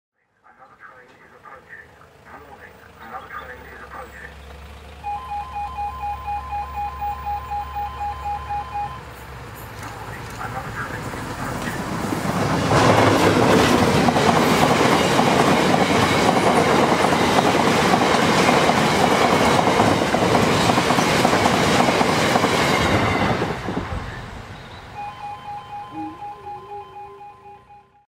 Debranded 59004 rumbles through Kimbridge with 7V16 for Whatley. (27.05.2014)
Still wearing the colours of Foster Yeoman, although with no branding, 59004 races through Kimbridge working 7V16 Fareham to Whatley empty stone wagons.